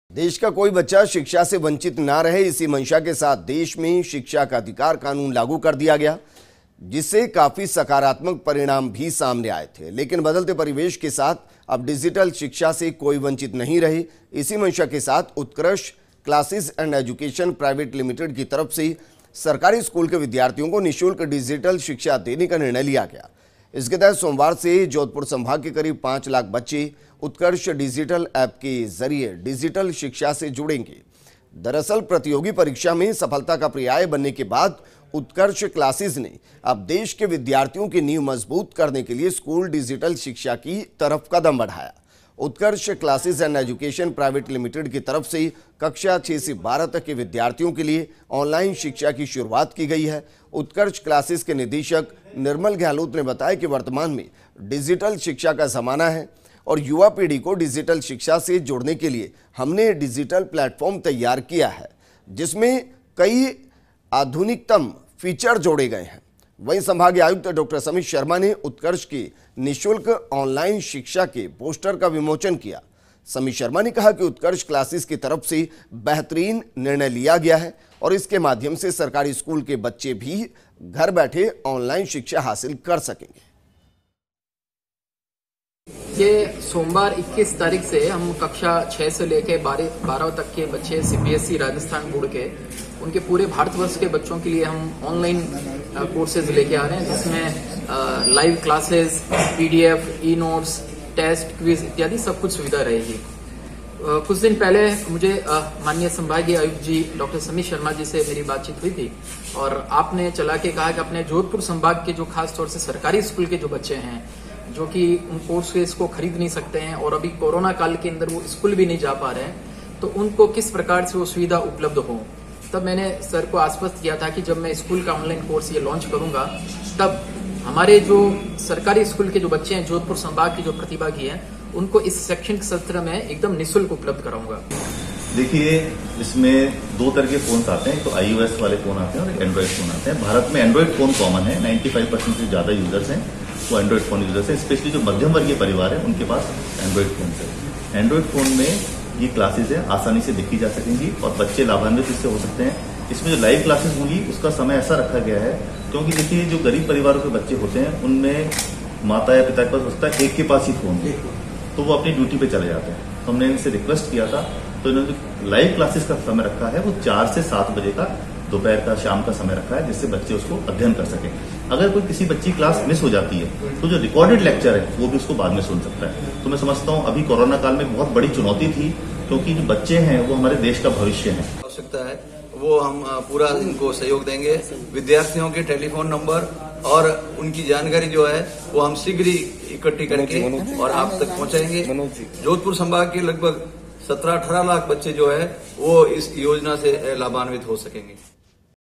Utkarsh Classes करीब 5 लाख बच्चों को देगा निशुल्क डिजिटल शिक्षा
About this Video:
Utkarsh Classes to provide free digital education to about 5 lakh children
About Channel: